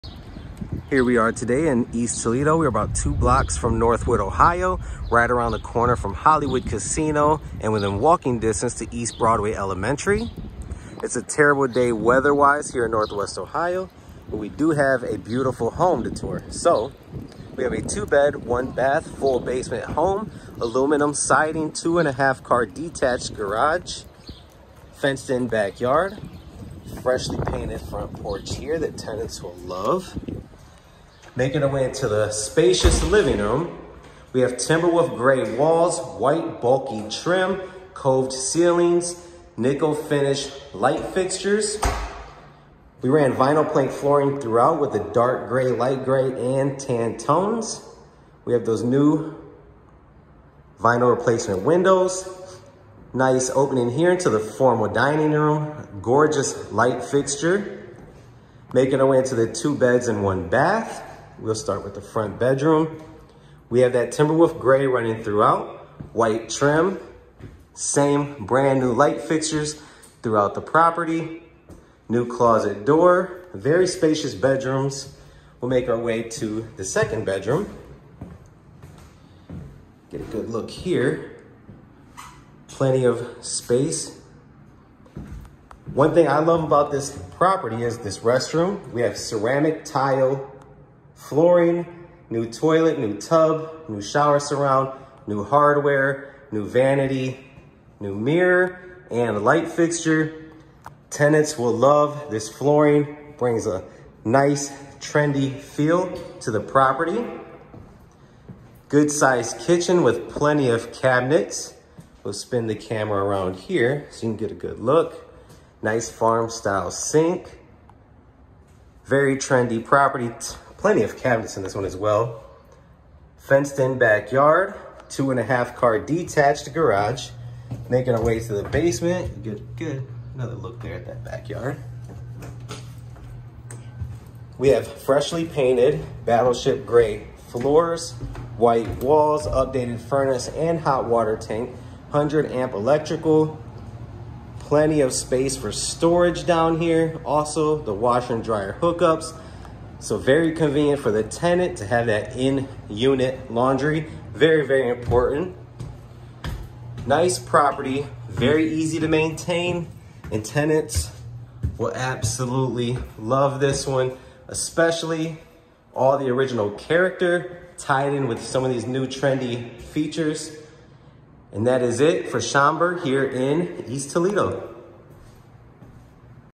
1658 Schomberg Toledo Ohio 43605
This updated 2 bedroom, 1 bathroom East Toledo home, has plenty to offer! Aluminum siding, vinyl replacement windows, new flooring throughout, new light fixtures, fresh paint throughout, beautiful ceramic tile flooring in the bathroom, new vanity, tub, shower surround, and hardware. Full basement and 2 car detached garage.